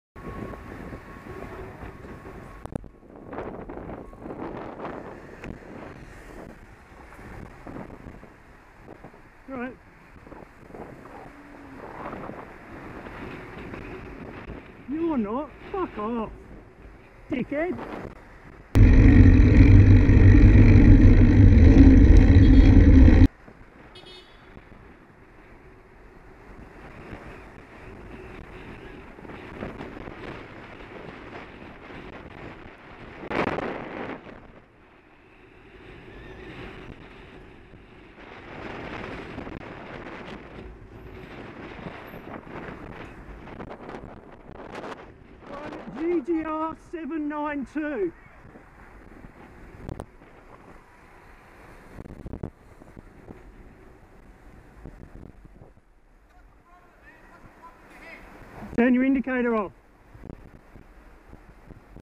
1GGR792 Tries To Push Into Lane, Fails 1st March 2019
Finding yourself in the wrong lane is no reason to side swipe a cyclist.
1GGR792
1GGR 792